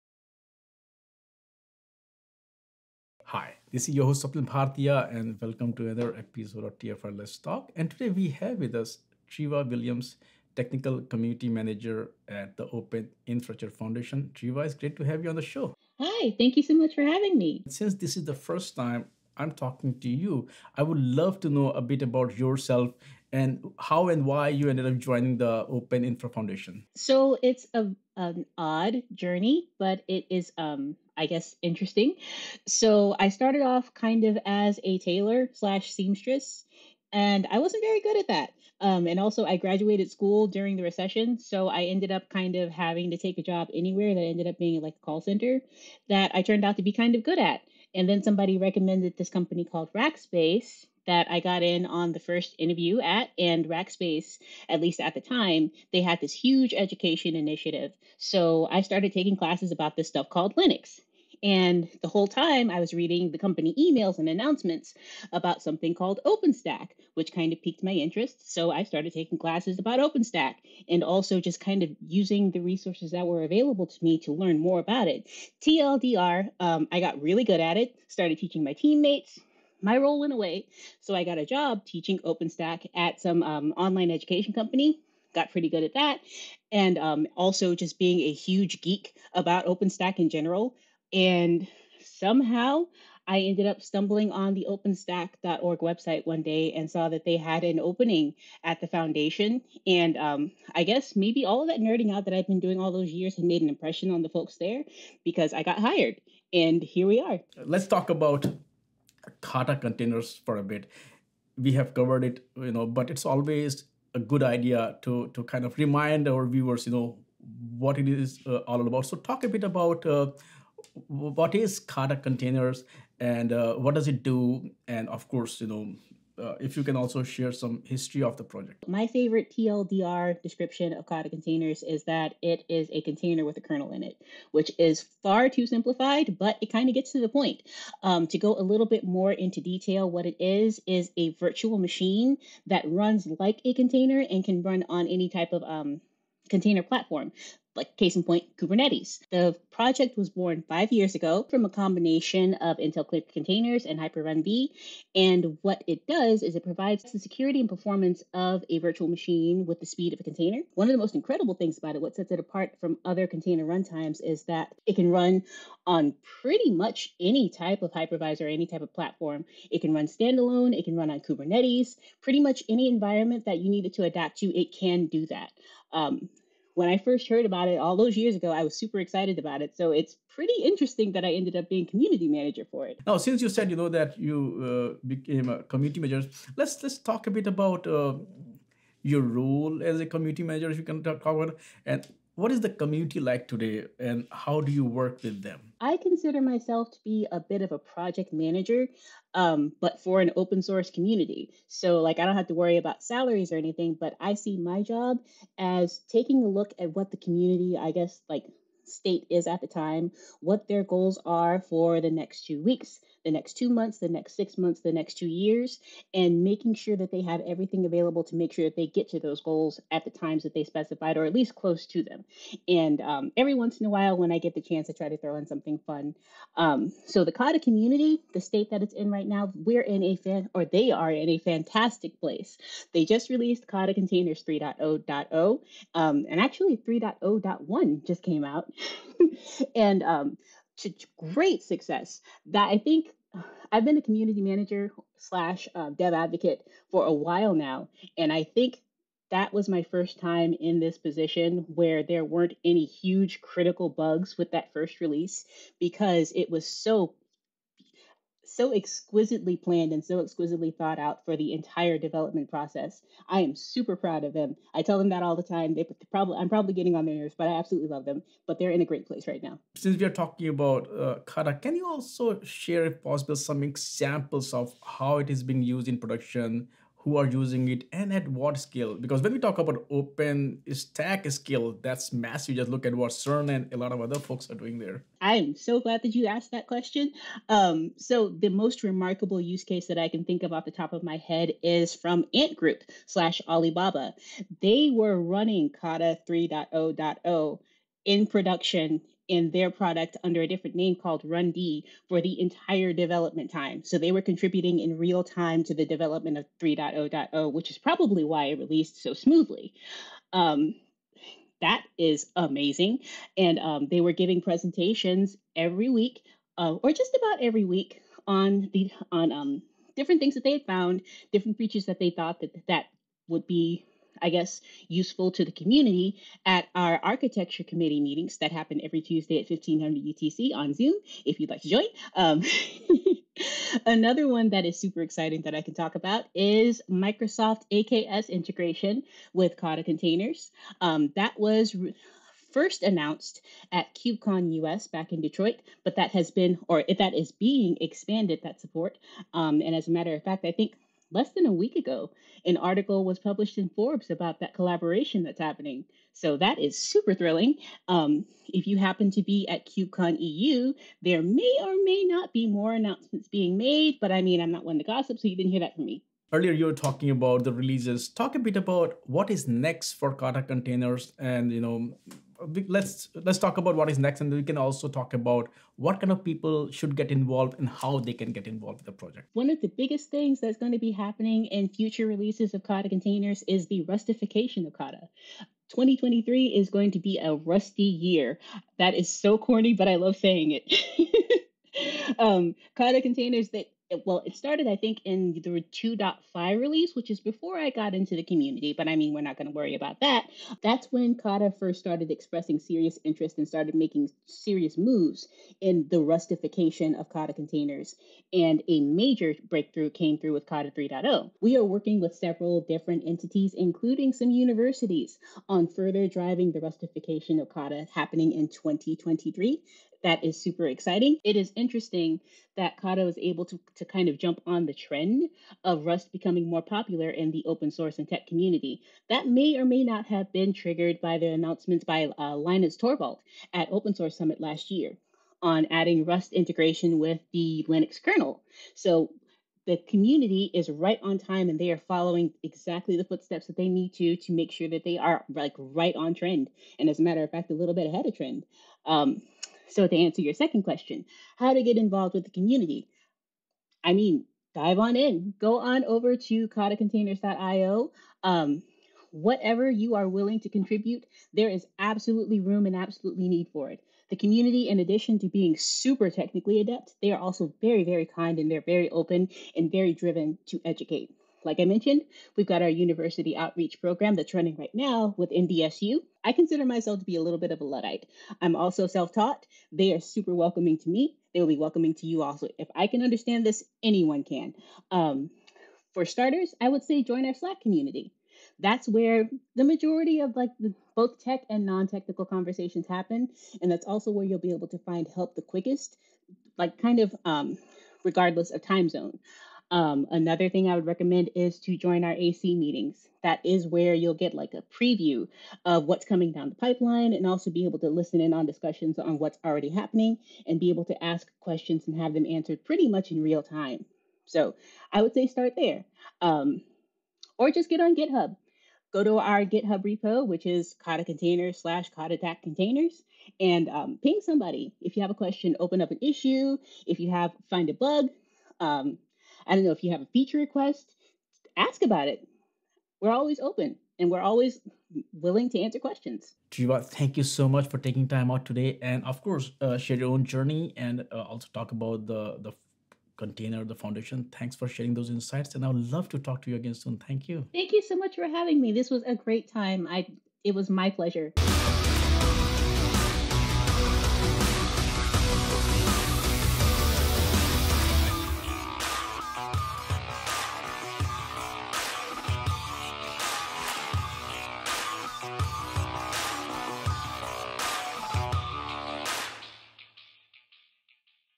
2023 To Be A Rust-y Year For Kata Containers | Treva Williams
Guest: Treva Williams
Foundation: OpenInfra Foundation

With the recent release of Kata Containers 3.0.0, one of the key goals has been to create a secure container runtime without sacrificing performance. One of the ways they are achieving this is with the Rust-ification of Kata components.

In this episode of TFiR: Let’s Talk, Swapnil Bhartiya sits down with Treva Williams, Technical Community Manager at Open Infrastructure Foundation (OpenInfra Foundation), to discuss Kata Containers and the recent 3.0.0 release. She goes on to talk about the community around Kata Containers and some of the notable use cases. Williams also talks about the progress they are making with Rust and what is next for the project.

Key highlights from this video interview are: